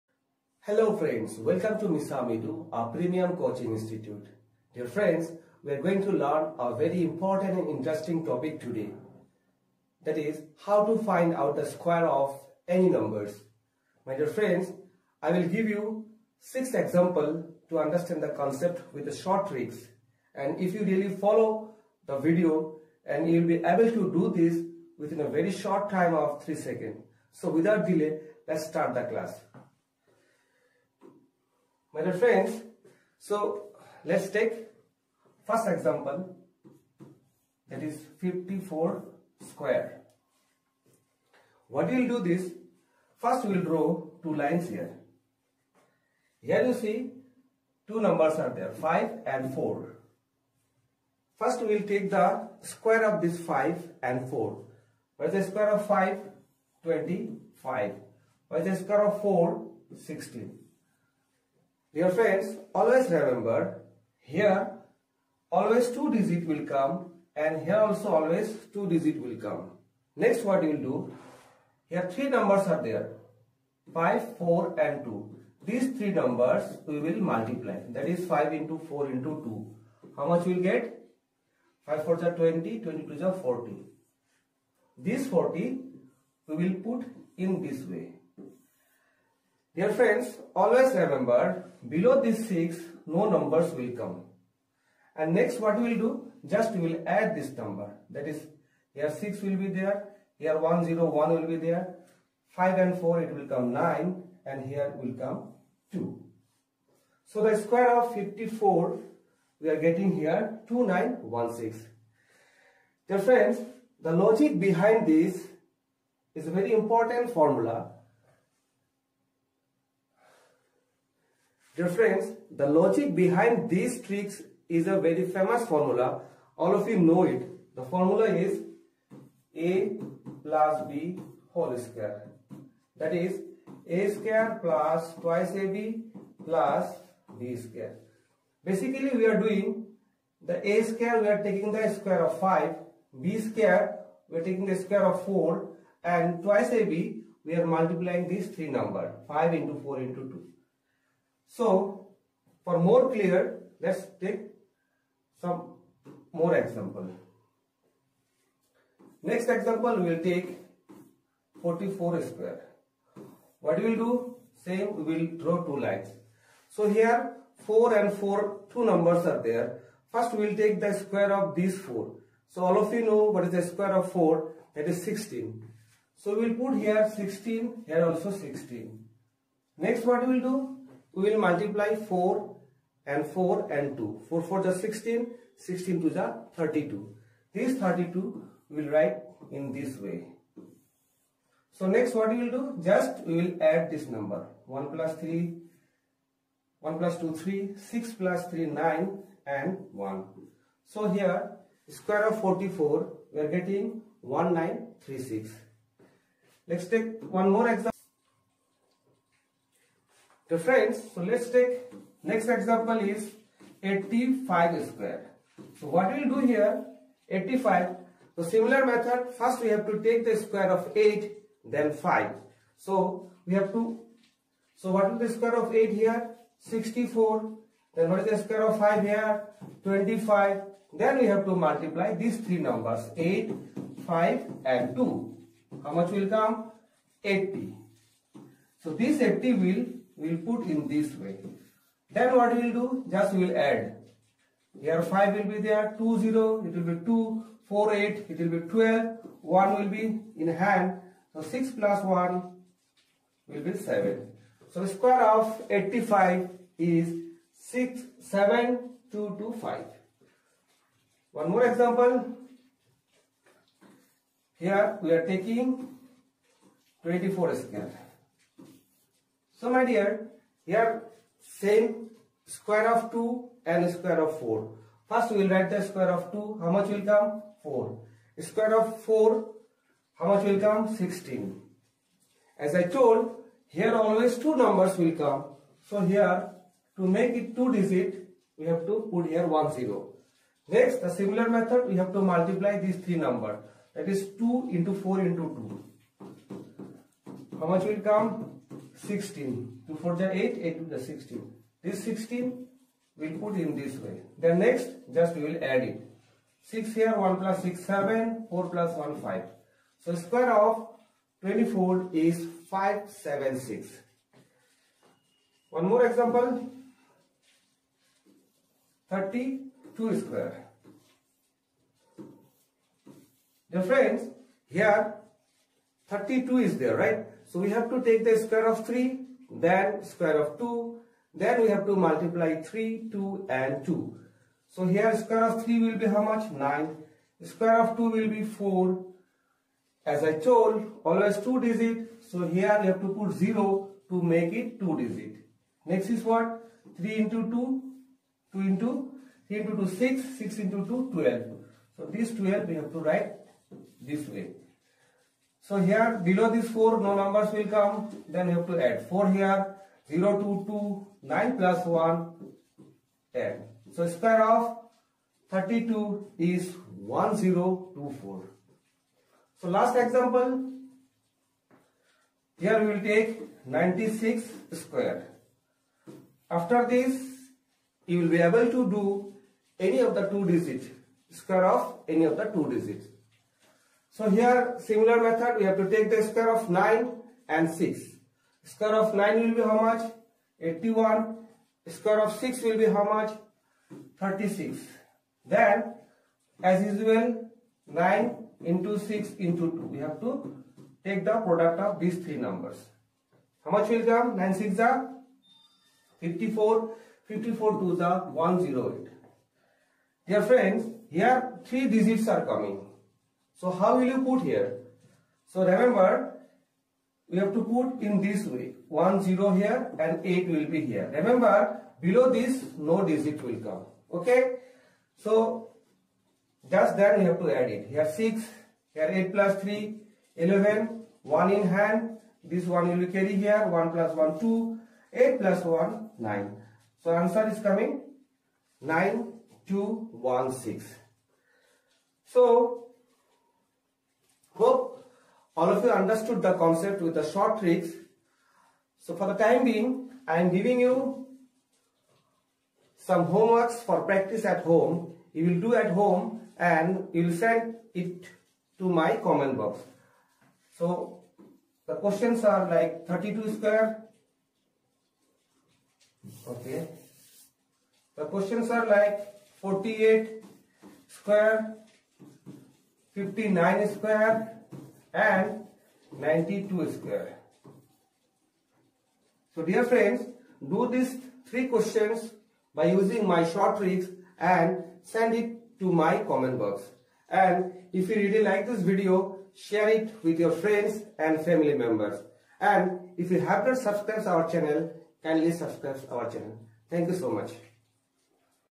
How to find Square of any two digit number with short tricks| part :-01|Ahmadur Rashid
In this video it is discussed about how to find square of any two digit number  for assam police AB UB and SI written exam.This video is also helpful for class eight nine ten and any other competitive Exams.